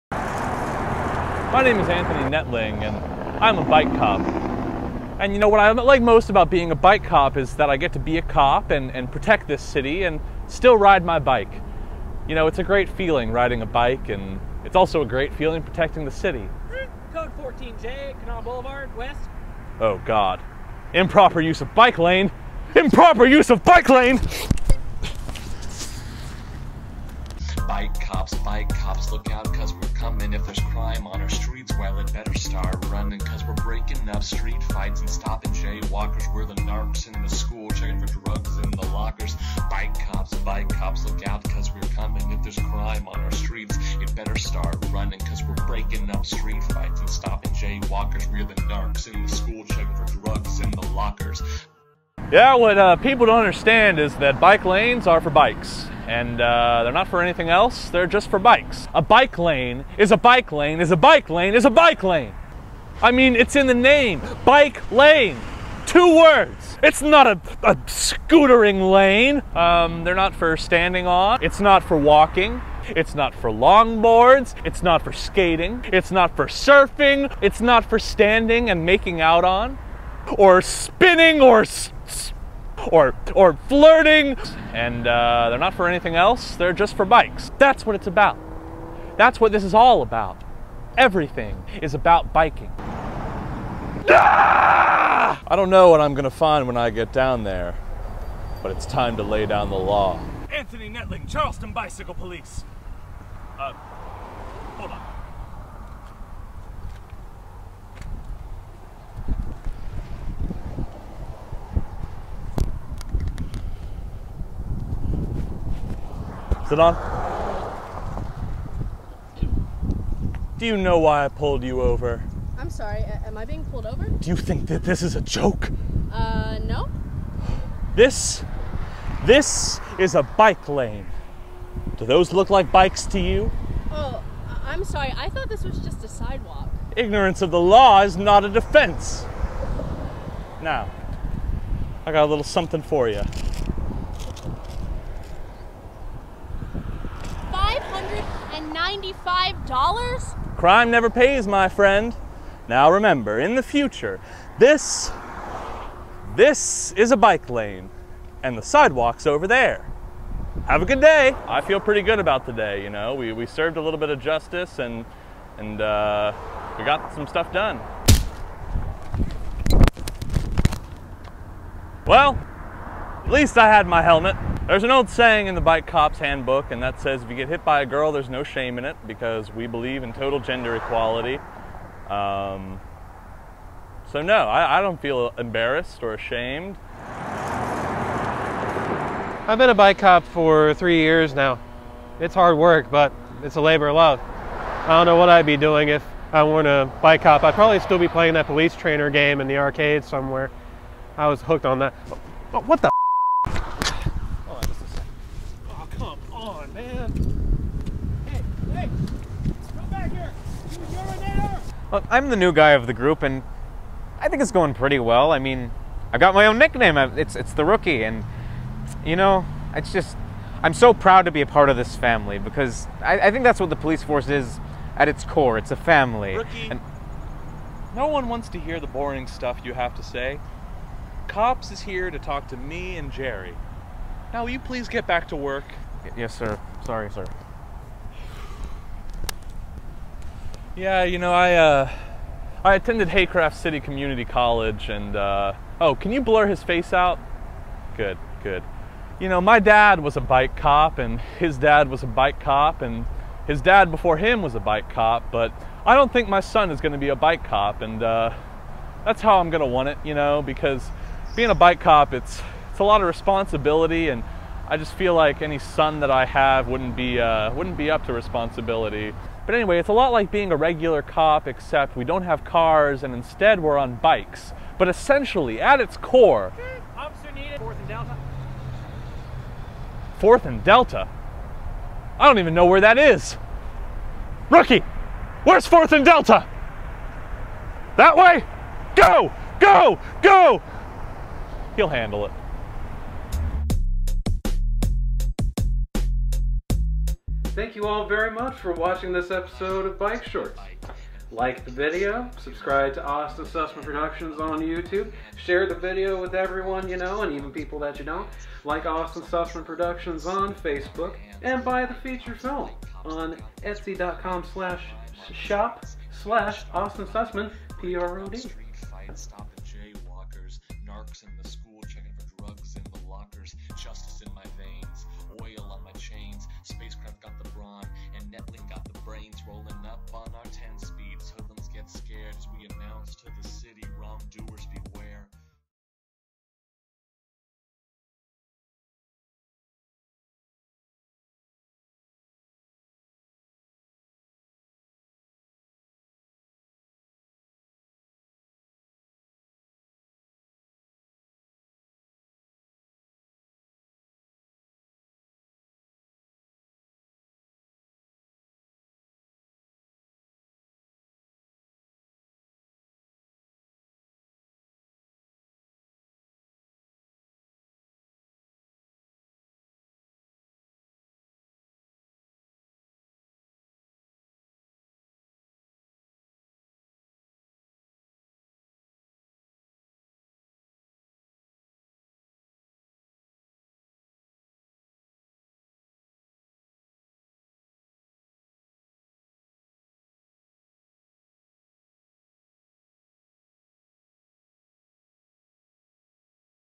bike COPS (Bike Shorts Episode 4)
A Parody of COPS featuring the characters from Austin Susman Productions' Bike Cops of Haycraft City. Written by Austin David Susman, with Story Consultant Indigo Rancourt. Guest Starring Caitlin Moore and Brett Smith.

Based on the Award Winning Feature Film "Bike Cops of Haycraft City," "Bike Shorts" is a series of short films featuring the continuing adventures of Anthony Nettling and Jerry Spacecraft.

To purchase the original award winning feature film "Bike Cops of Haycraft City"